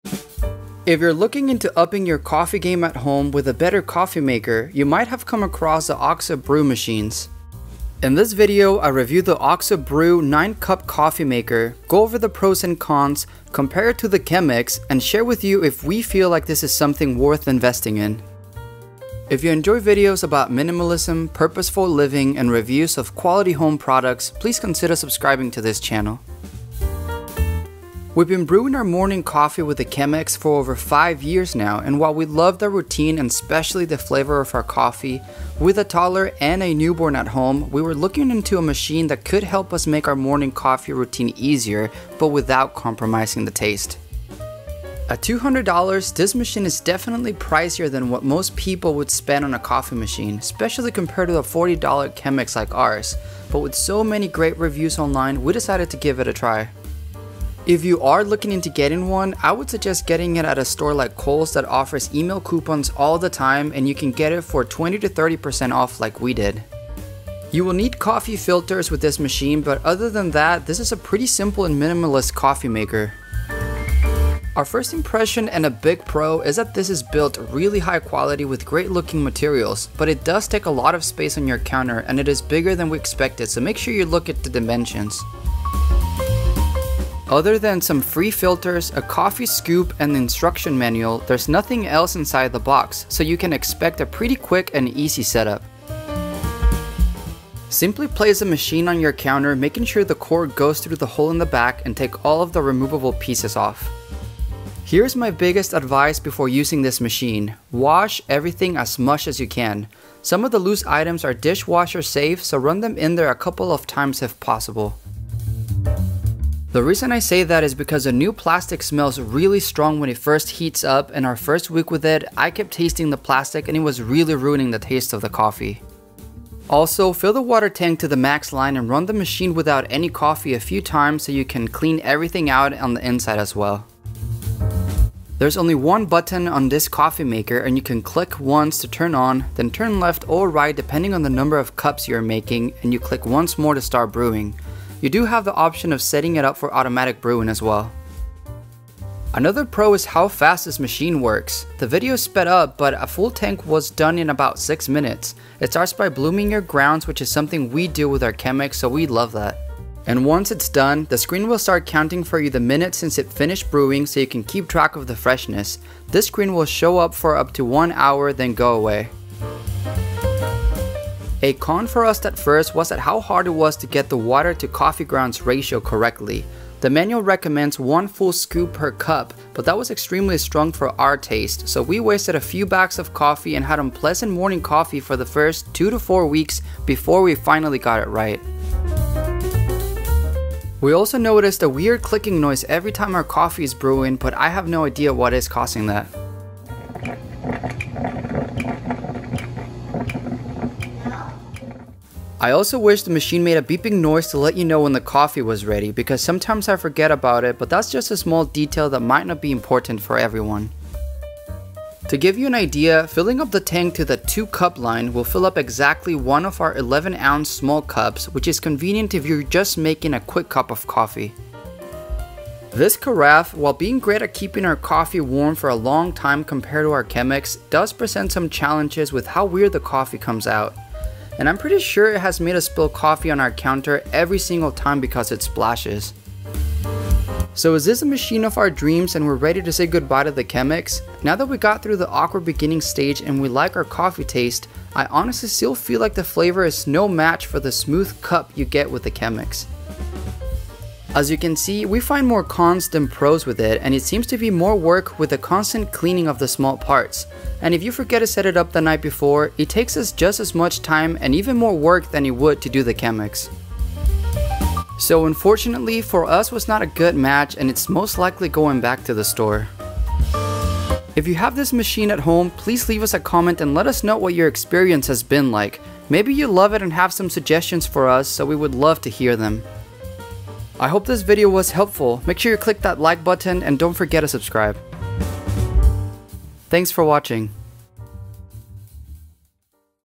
Oxo Brew-9 cup Coffee Maker Review - Better Than Chemex?
In this video I review and go over the features of the Oxo Brew 9-cup coffee maker and compare it to the Chemex since it's meant to brew your coffee similarly. Is it worth the high price, though?

Mindful Home